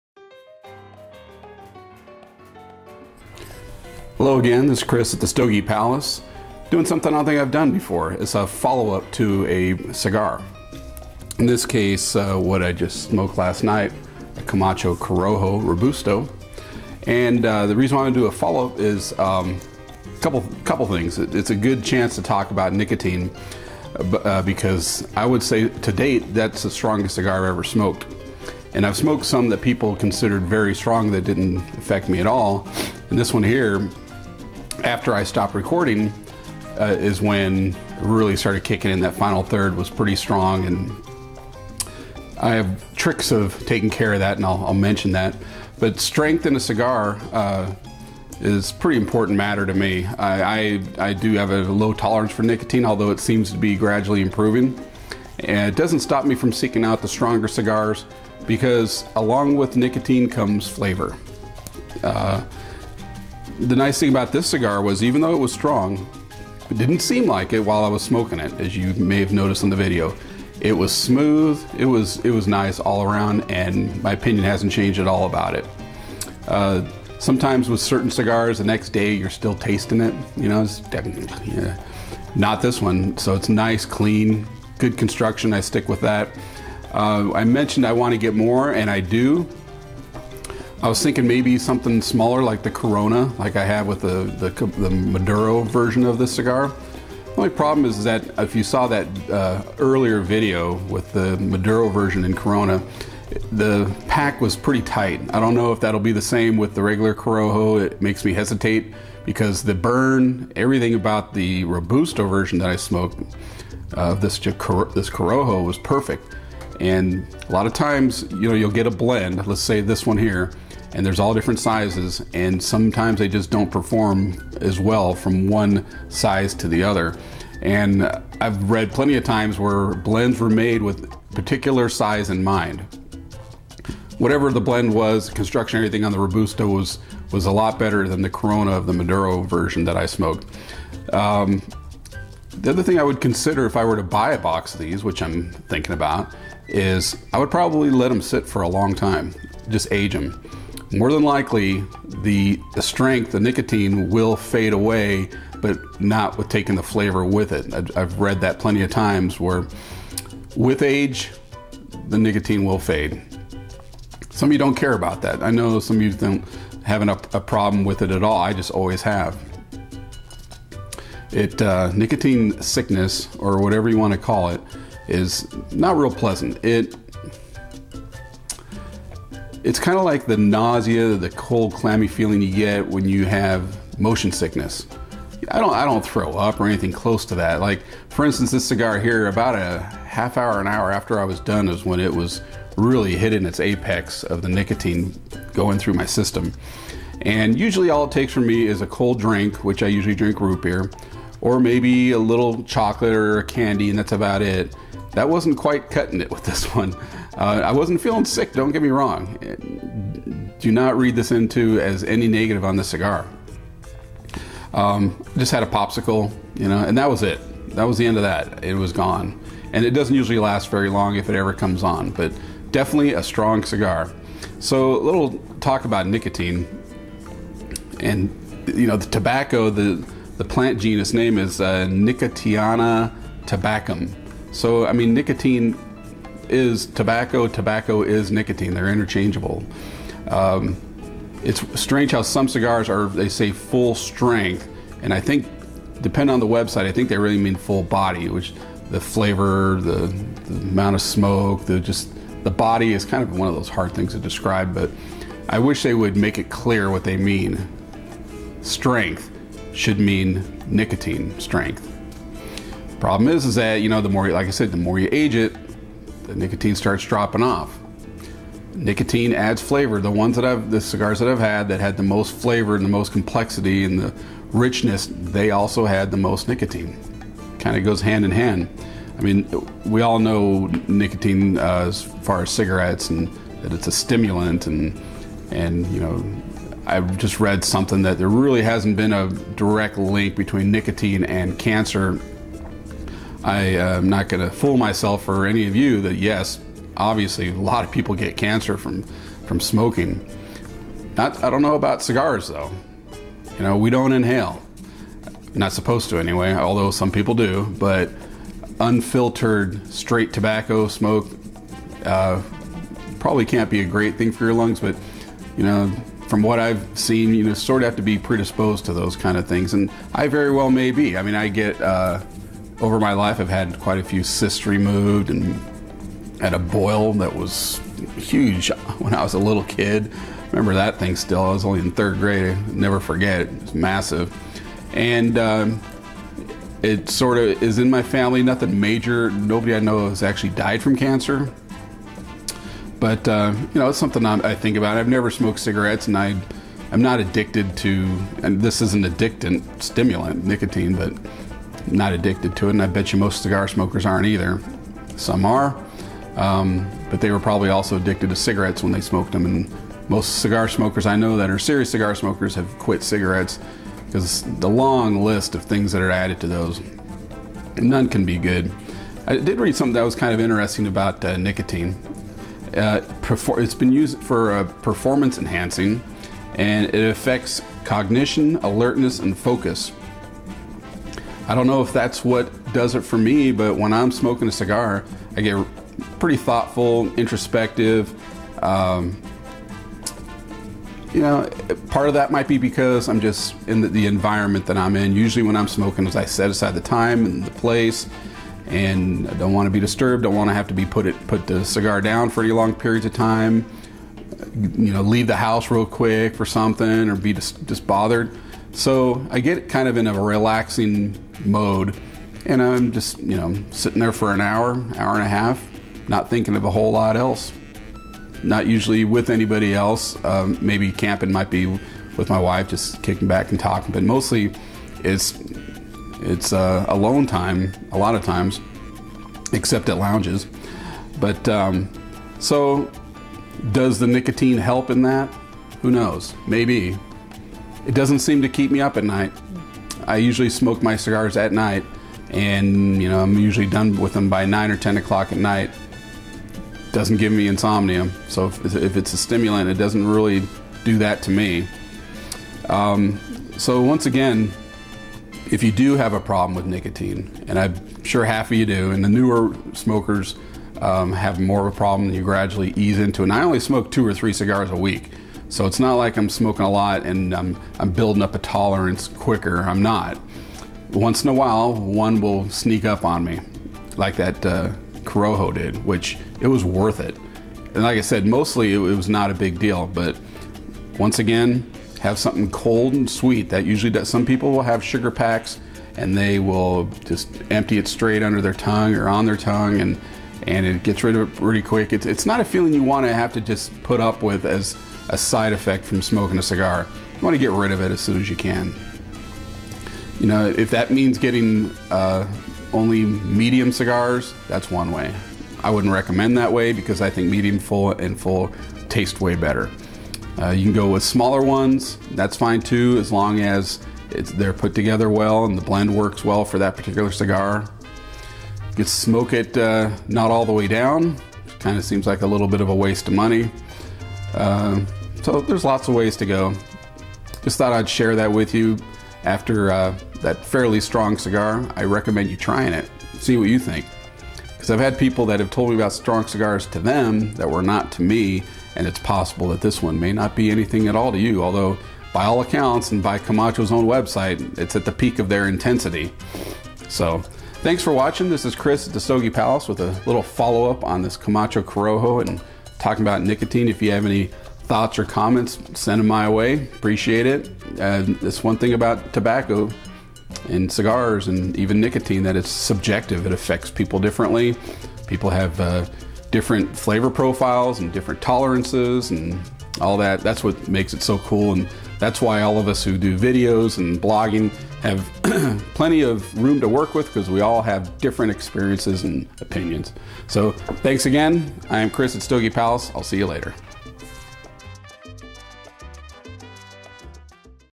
Camacho Corojo follow-up and thoughts/tips on nicotine
Here I give a rare follow-up to my previous cigar review, in this case the Camacho Corojo Robusto.  In that vein, I talk about nicotine and its effects.